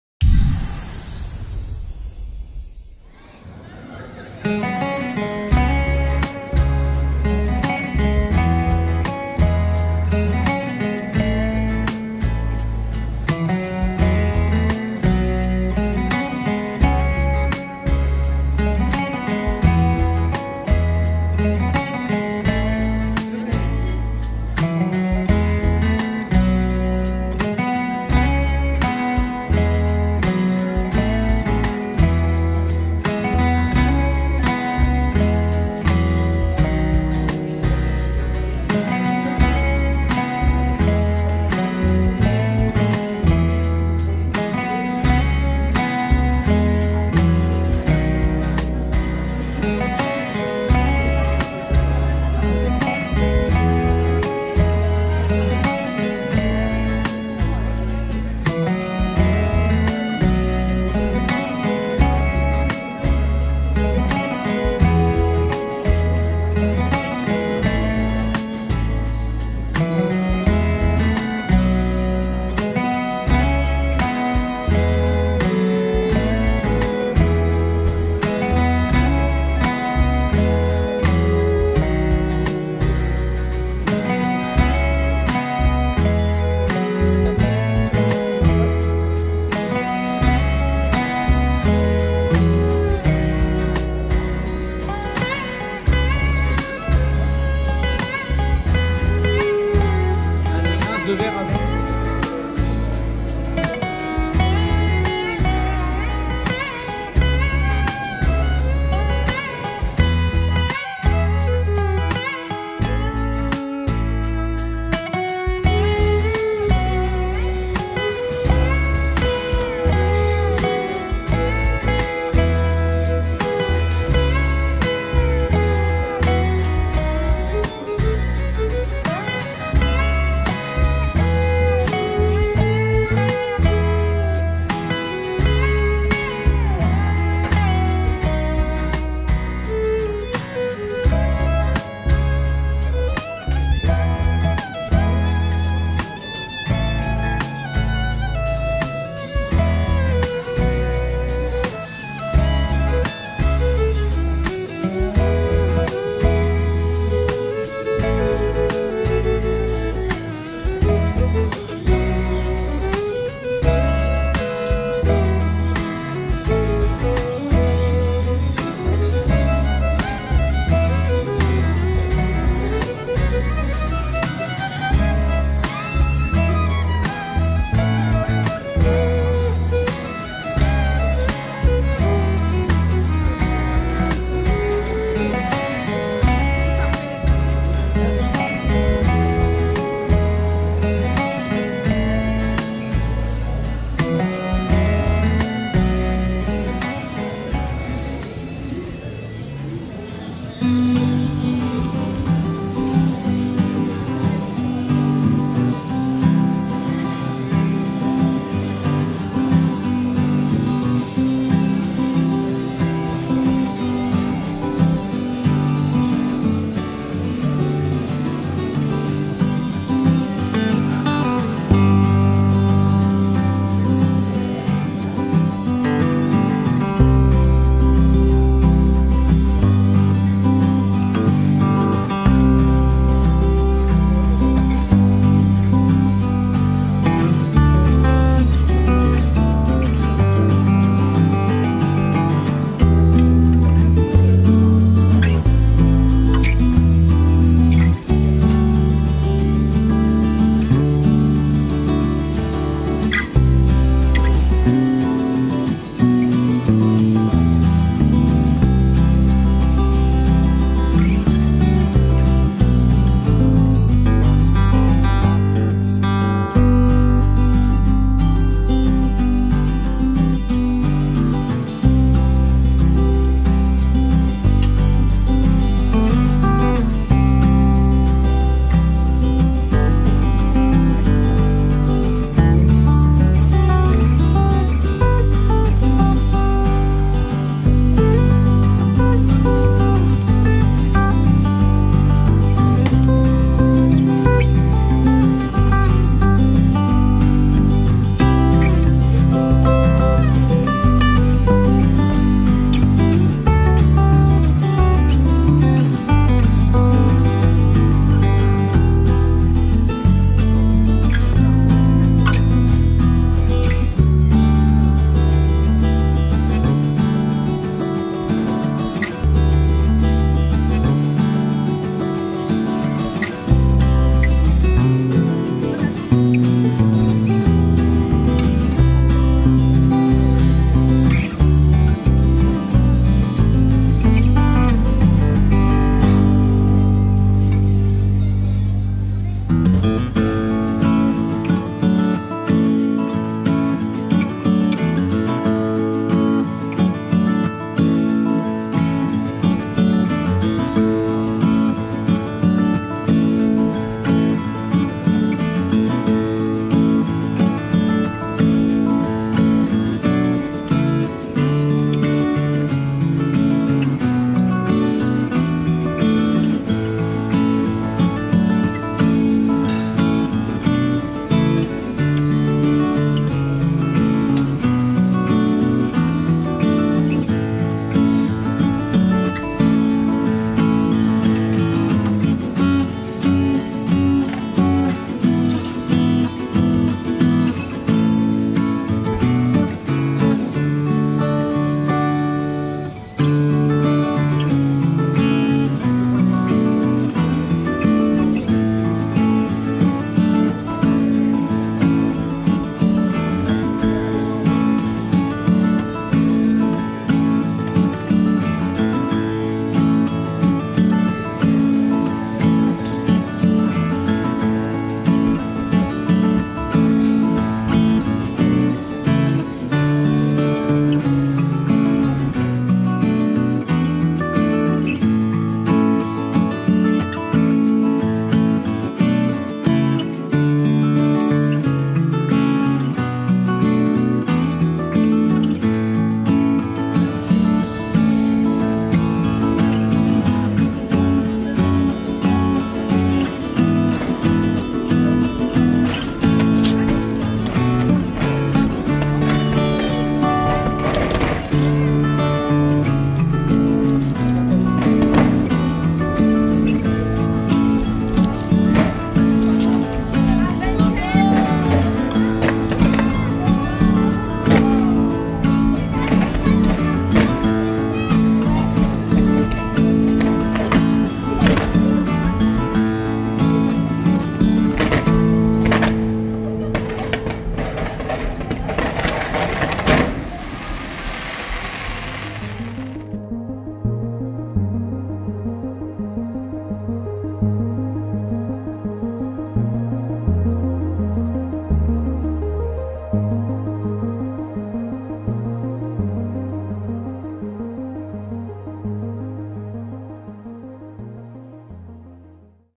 Billy Bob's Buffet - Disney Village - Disneyland Paris
Discover the Billy Bob's Buffet at Disney Village, open since October 22, 2023 (€40 / Adult).